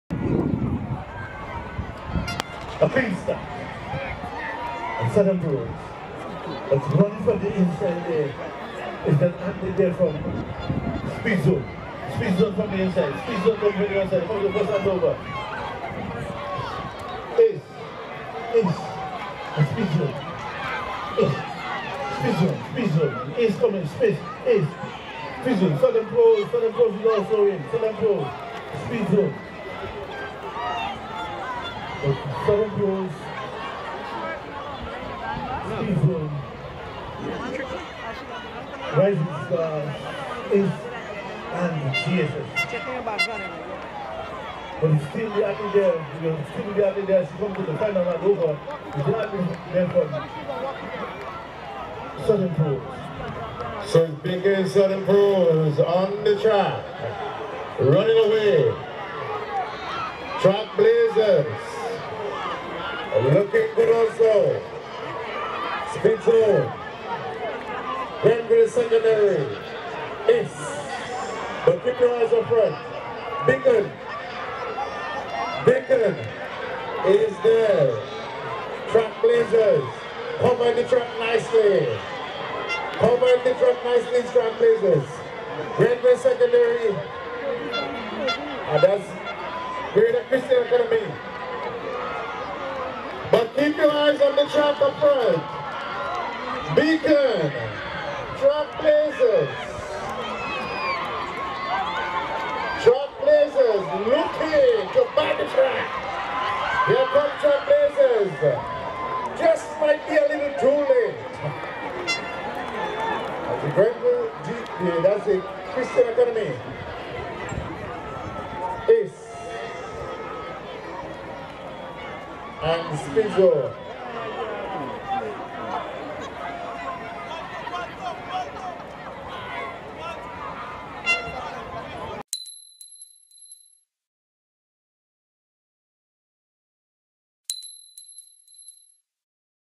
Twilight Relays Under 18 Plus Girls 800m Medley
Event 23 Girls 800 Sprint Medley 1x1x2x4 18 Plus

Finals

1 St. Davids Track A 1:47.30
2 Beacon Southern Pros A 1:48.30
3 Grenada Christia A 1:55.00
4 Ace A 1:58.30
5 Speedzone A 2:07.20